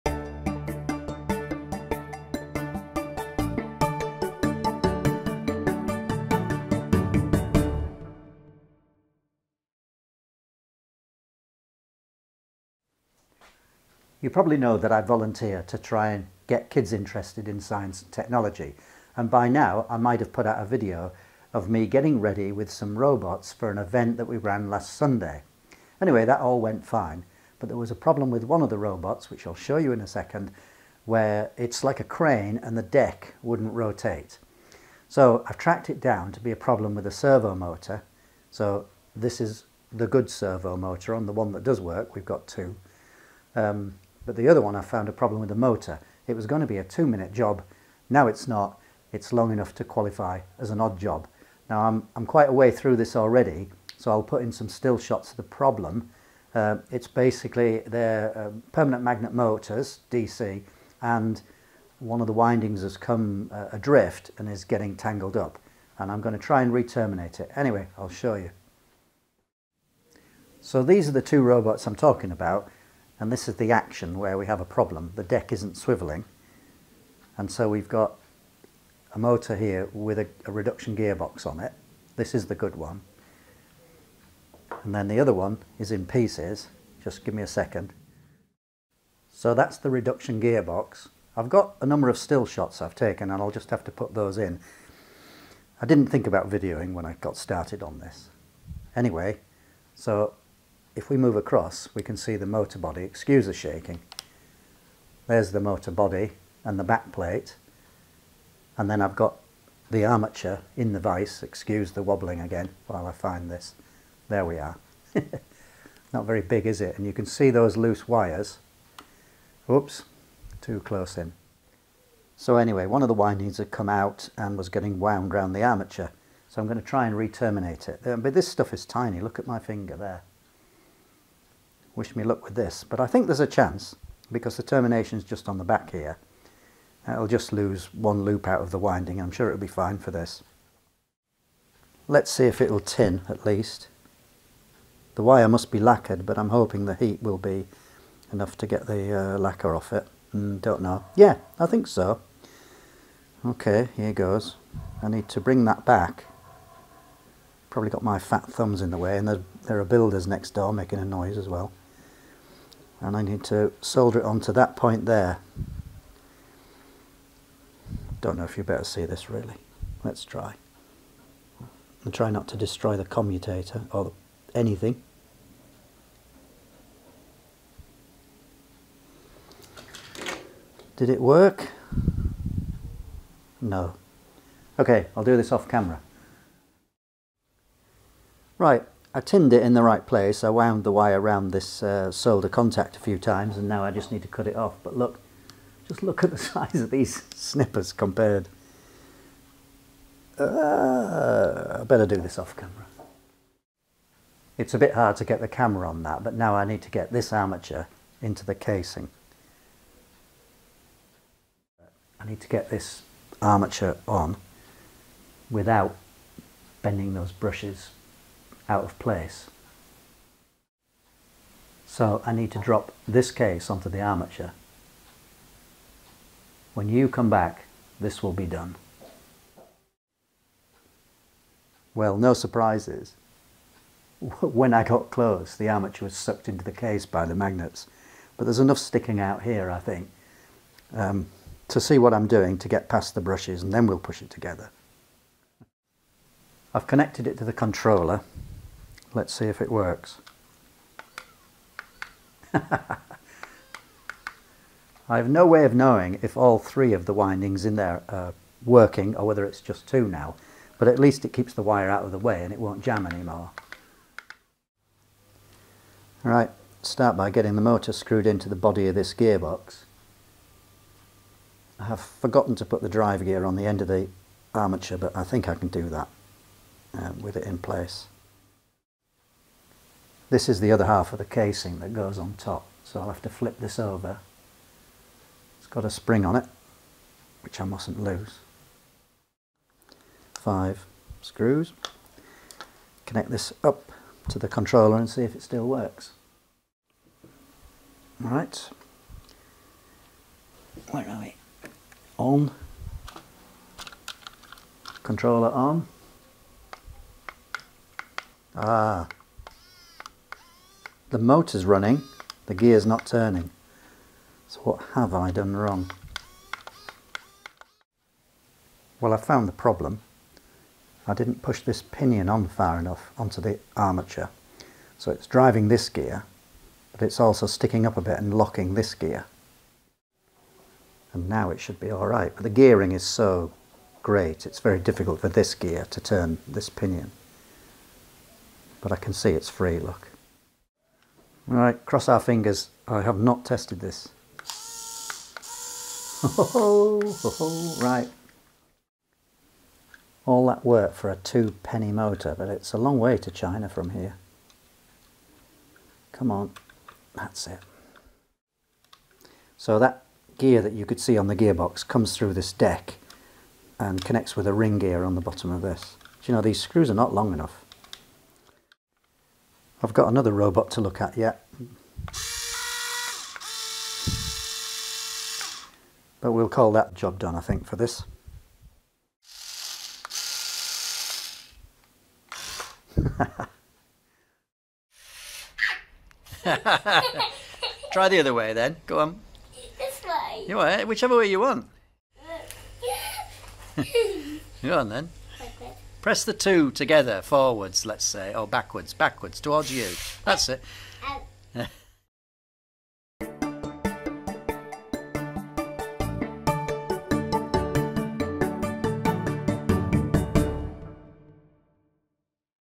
HS149 Robot small DC motor fix
Here I’m fixing one of the small DC motors from the educational robot crawler crane.